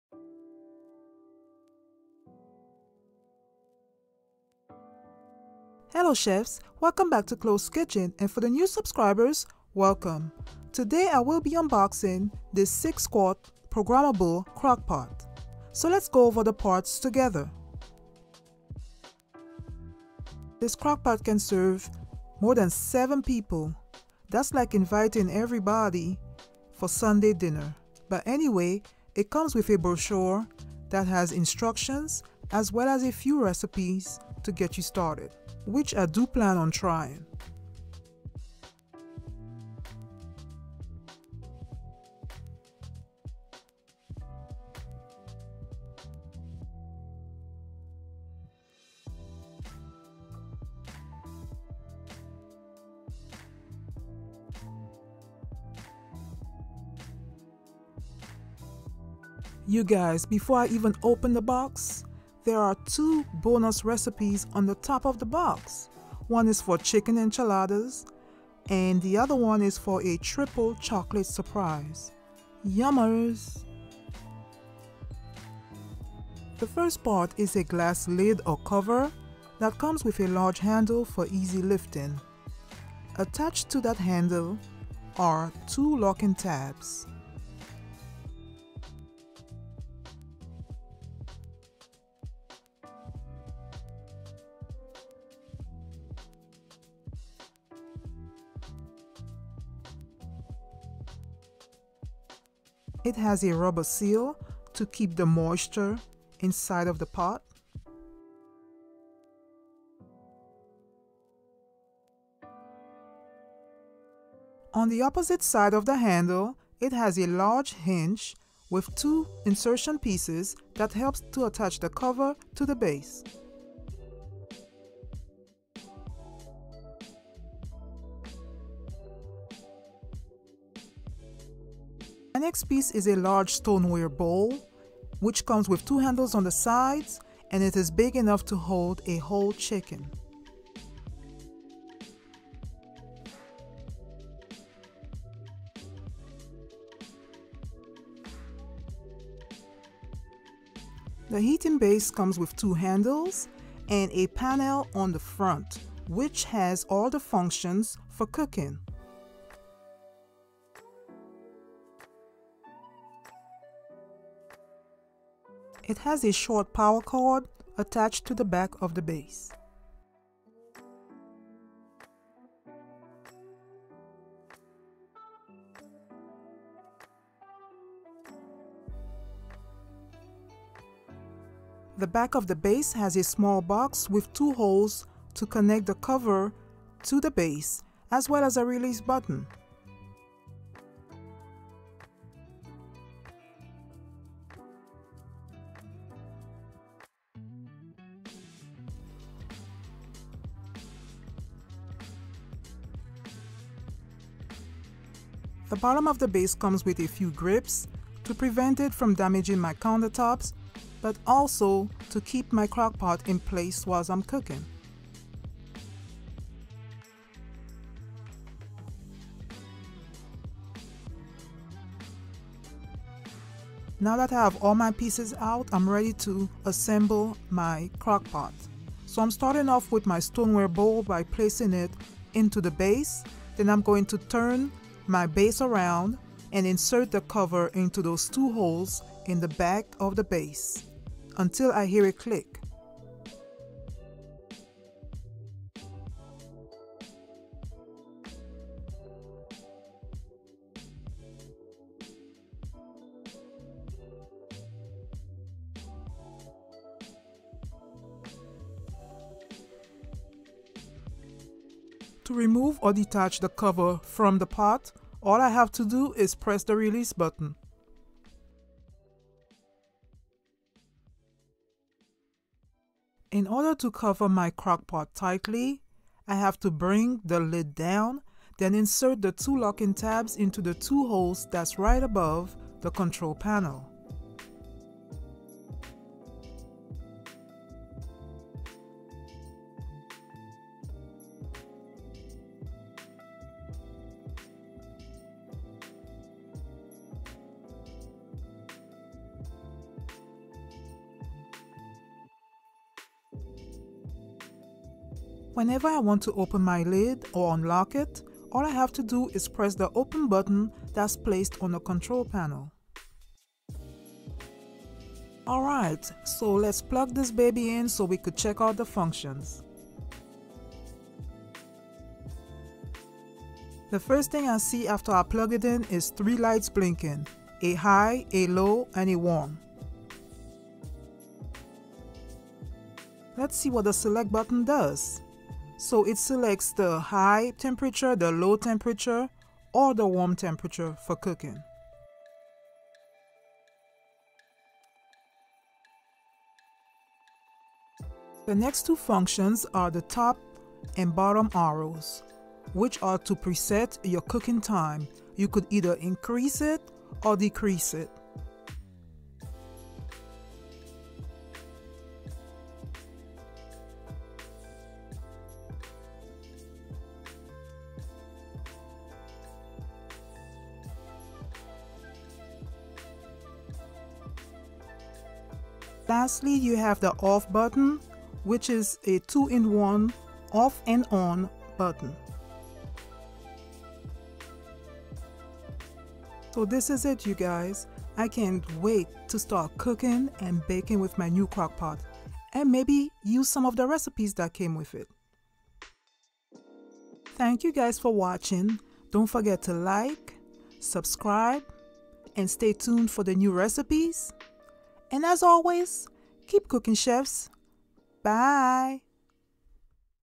6 QUART CROCKPOT SLOW COOKER UNBOXING
This "6 QUART CROCKPOT SLOW COOKER UNBOXING" will show you the different functions. This"6 QUART CROCKPOT SLOW COOKER UNBOXING" is great for large family meals and events.

Music Provided to me Courtesy of Youtube Library

This product is not sponsored, I purchased this item with my own money.